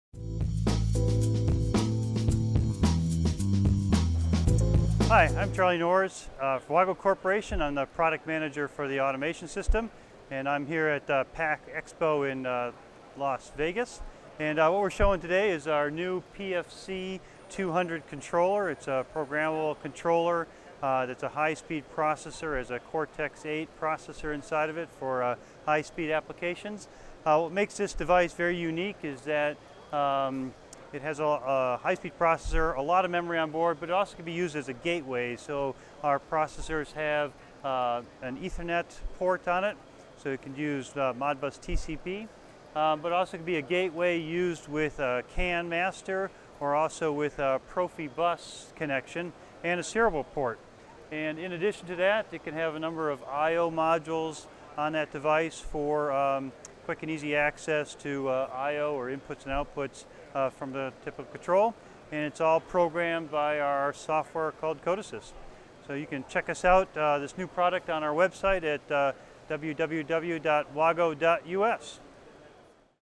Wago - Pack Expo 2013 - PFC 200 Controller
Wago shows Design World their PFC 200 Controller at Pack Expo 2013 in Las Vegas